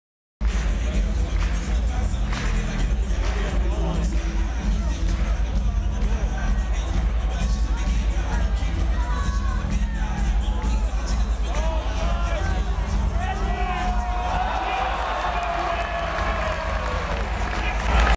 Warriors halftime
Cool dunking tricks with trampoline

(Mostly just uploaded this video to test out my camera's wifi features. Don't be surprised if this video is gone soon.)